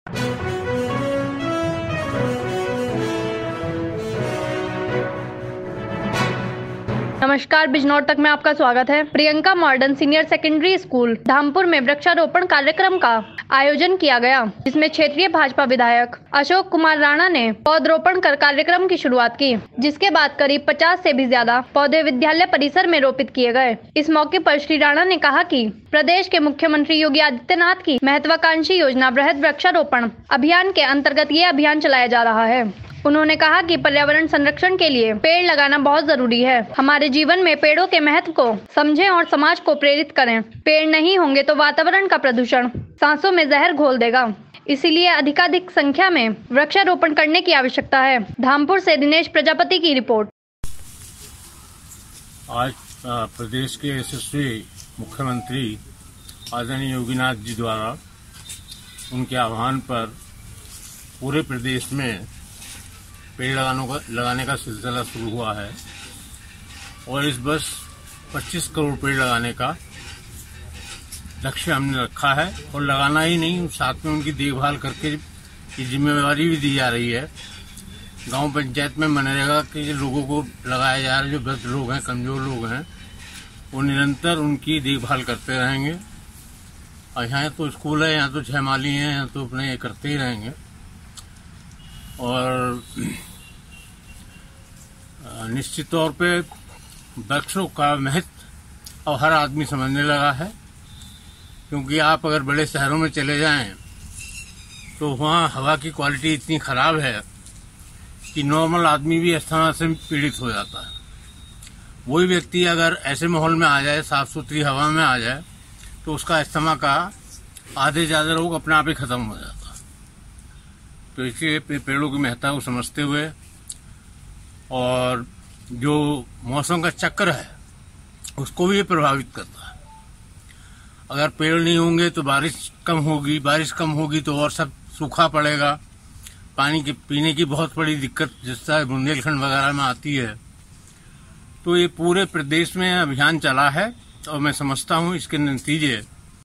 BIJNOR TAK || धामपुर-अशोक राणा ने किया कार्यक्रम का शुभारम्भ
धामपुर। प्रियंका मॉडर्न सीनियर सेकेंडरी स्कूल धामपुर में वृक्षारोपण कार्यक्रम का आयोजन किया गया, जिसमें क्षेत्रीय भाजपाविधायकअशोककुमारराणा ने पौधारोपण कर कार्यक्रम की शुरुआत की। जिसके बाद करीब 50 से भी ज्यादा पौधे विद्यालय परिसर में रोपित किए गए। इस मौके पर श्री राणा ने कहा कि प्रदेश के मुख्यमंत्री योगी आदित्यनाथ की महत्वाकांक्षी योजना वृहद वृक्षारोपण अभियान के अन्तर्गत ये अभियान चलाया जा रहा है। उन्होंने कहा कि पर्यावरण संरक्षण के लिए पेड़ लगाना बहुत जरूरी है। हमारे जीवन में पेड़ों के महत्व को समझने और समाज को प्रेरित करें। पेड़ नहीं होंगे तो वातावरण का प्रदूषण सांसों में जहर घोल देगा, इसलिए अधिकाधिक संख्या में वृक्षरोपण करने की आवश्यकता है। धामपुर से दिनेश प्रजापति की रिपोर्ट